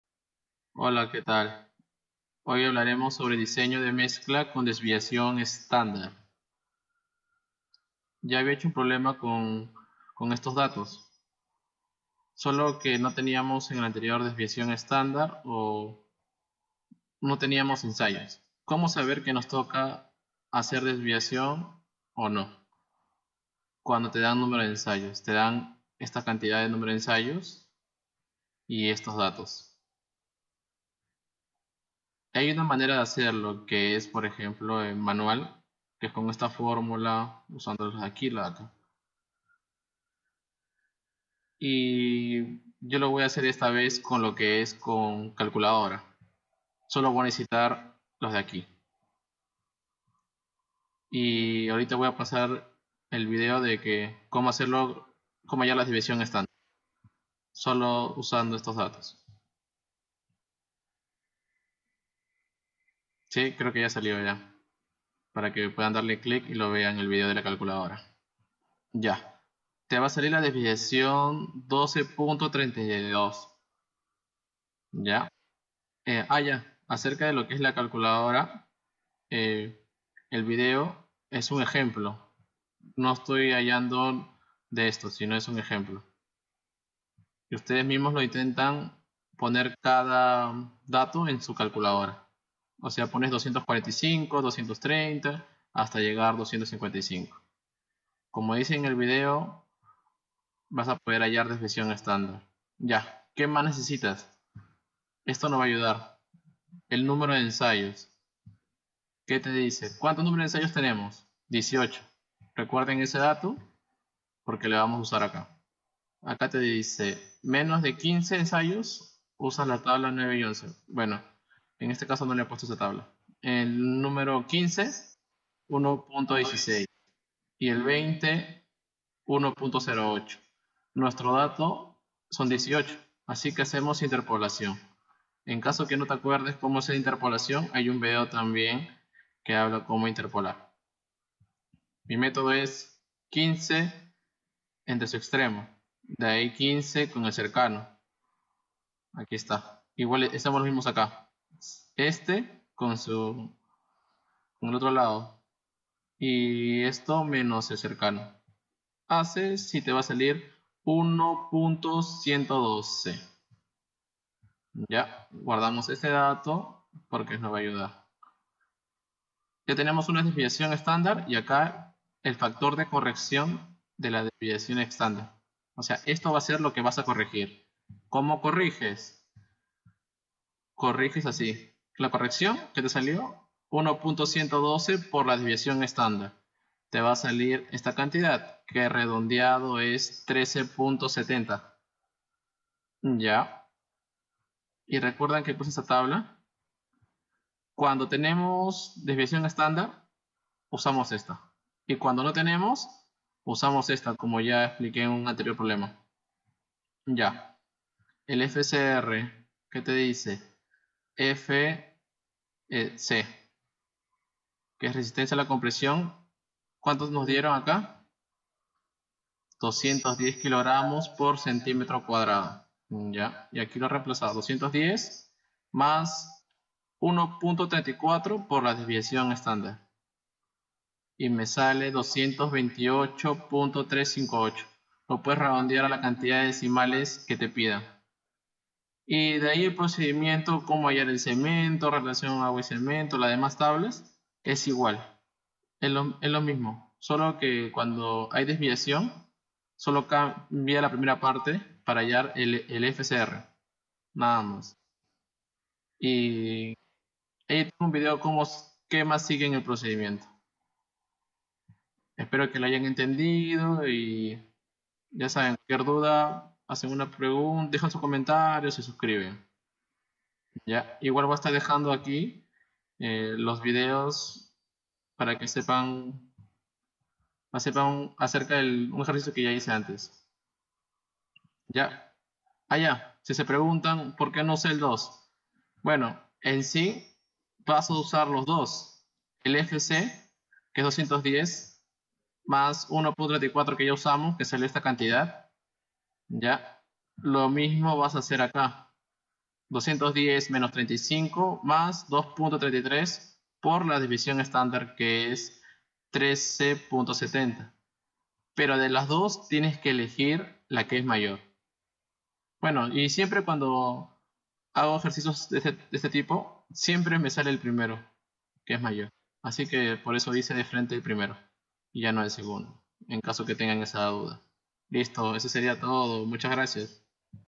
diseño de mezcla con desviacion estandar pc1 tecnologia de concreto 2020
Aqui enviare un video para usar calculadora para hallar  la desviacion estandar para luego corregirla, los pasos que siguen es lo mismo que mi anterior video.